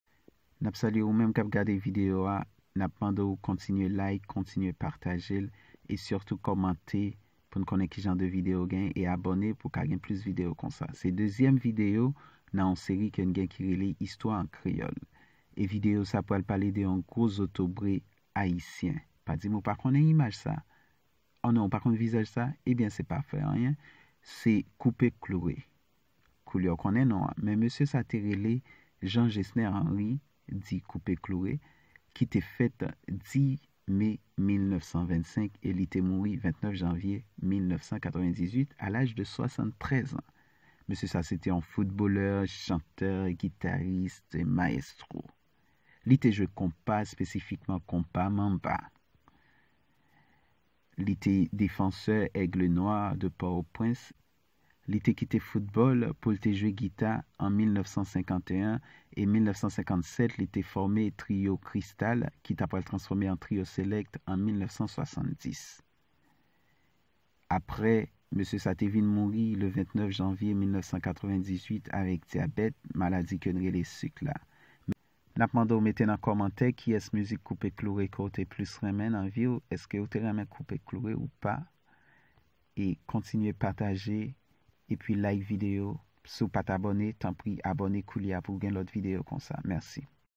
Koupe Kloure te Tebe oubyen Dyabetik? - Istwa an kreyòl #2
Video sa bay Key Bagay ou dwe konnen sou koupe kloure.